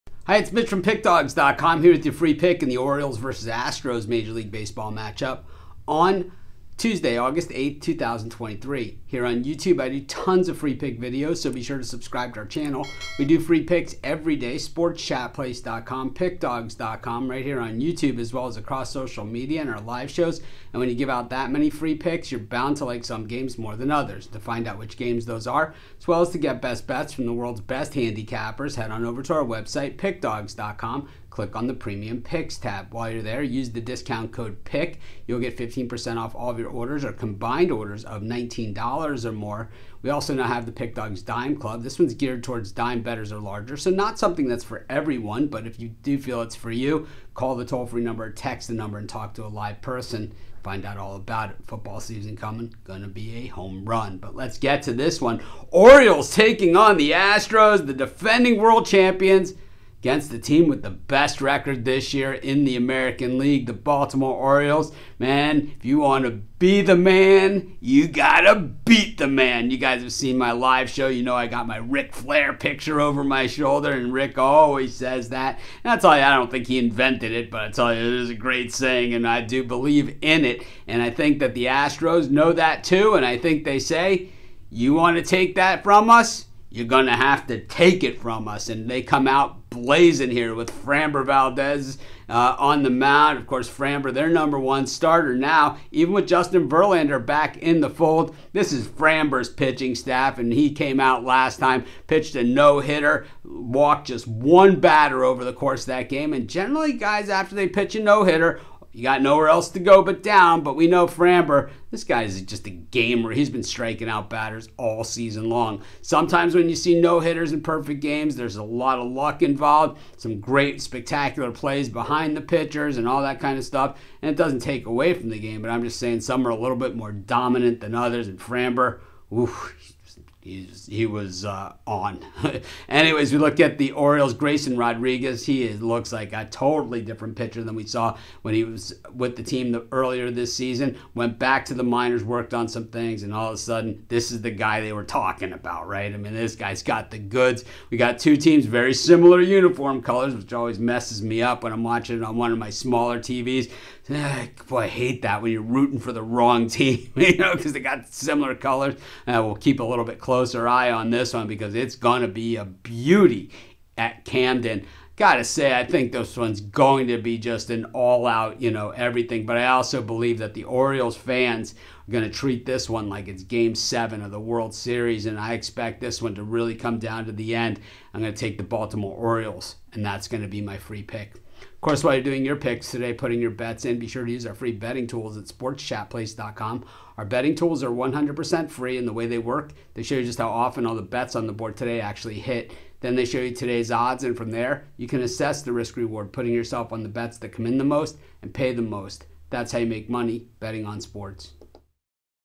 Baltimore Orioles vs Houston Astros 8/8/23 MLB Free Pick | MLB Betting Tips
The Houston Astros face off with the  Baltimore Orioles in Tuesday's MLB Baseball action. Get Mitch's Expert Pick and Prediction for this game.